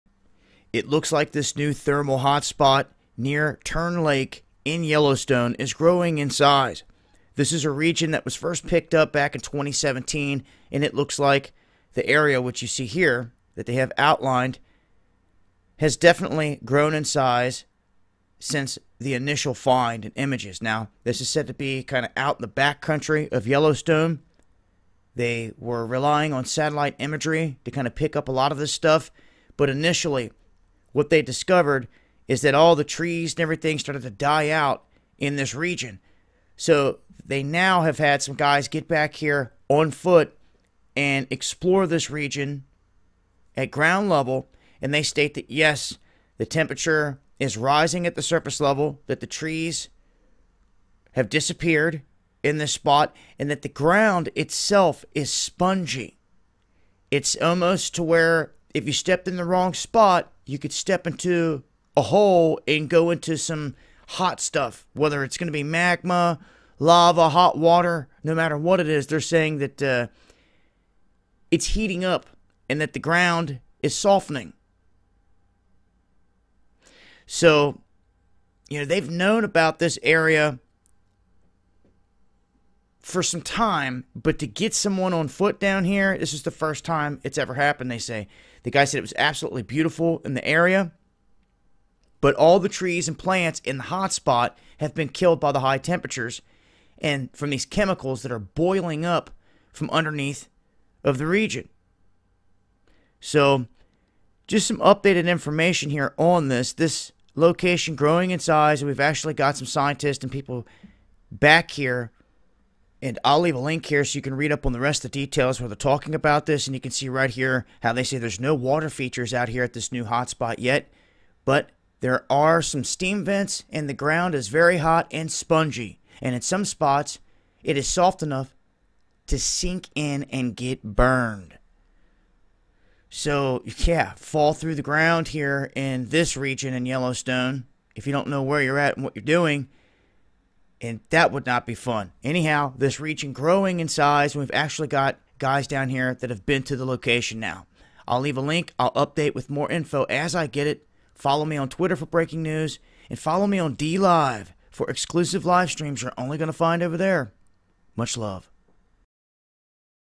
Yellowstone's New Thermal Area Is Growing, Ground Is Hot and Spongy
Yellowstone National Park is growing a new thermal area.

The new thermal area is so deep in the backcountry it took satellites to spot the telltale signs.

Jeff Hungerford, a Yellowstone National Park geologist, said the main signs they see are heat and carbon dioxide.

Researchers say there are no water features at the new hot spot yet, but they note that there are some steam vents. Also, the ground is very hot and spongy, and in some spots it is soft enough to sink in and get burned.

Bitcoin- 1EZdiSRfmPkGF7teAyUyoPotjzWYBtPwGn